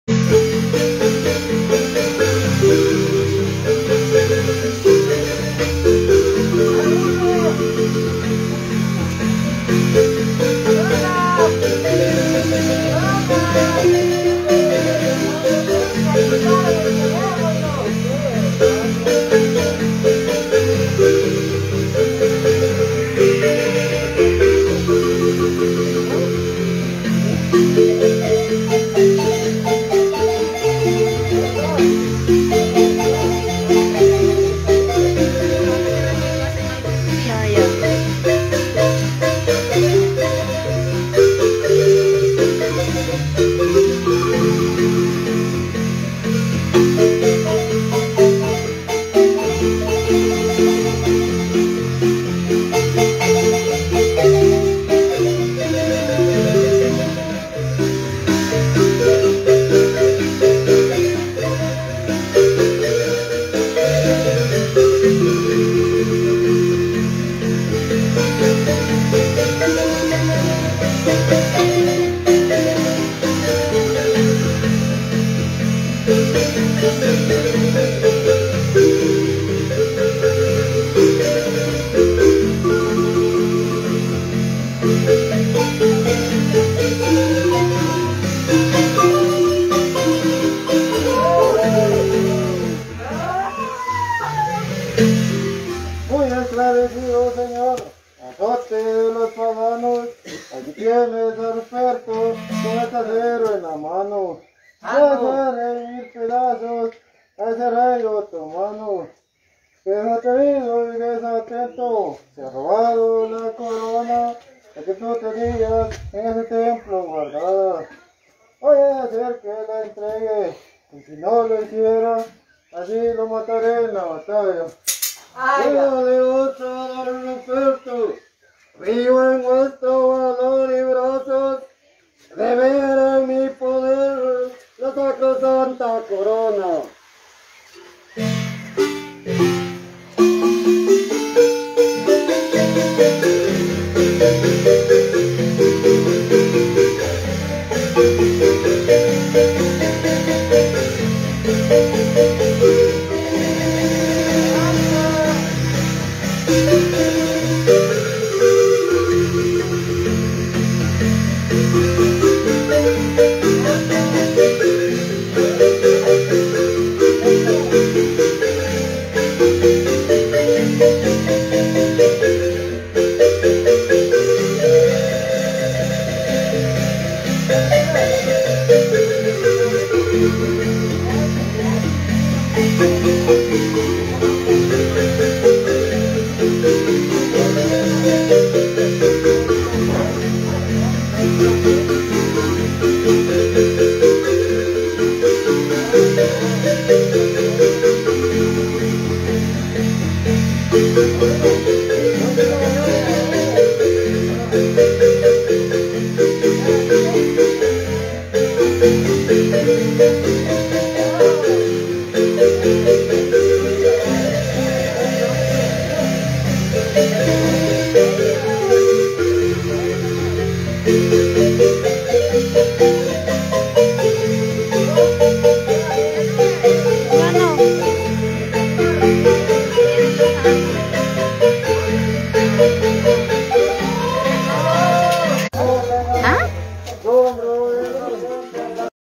El Baile de los Moros: Tesoro Cultural de Granados
La danza de los moros y cristianos es una tradición arraigada en varios departamentos de la República de Guatemala. Se celebra en diferentes fechas y fiestas patronales en cada municipio. Además, la práctica ha evolucionado hasta volverse un momento de diversión entre familia.